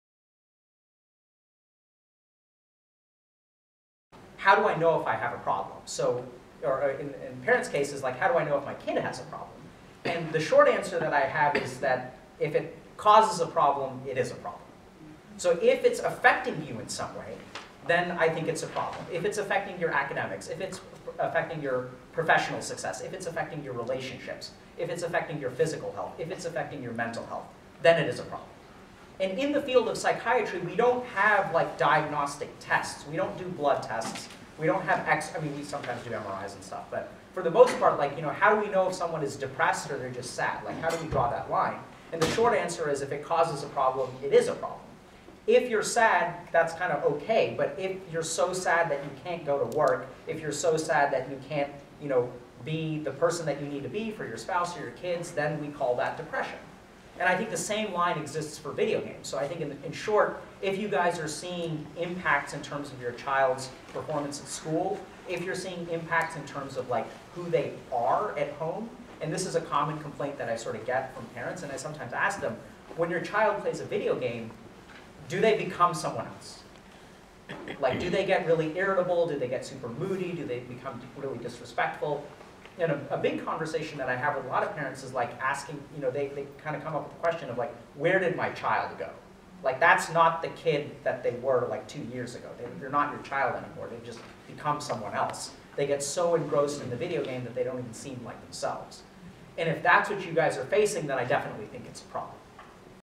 How Do You Know if You or Someone else Has a Problem with Video Games? | Episode 1
How do I know if my child has a problem with video games?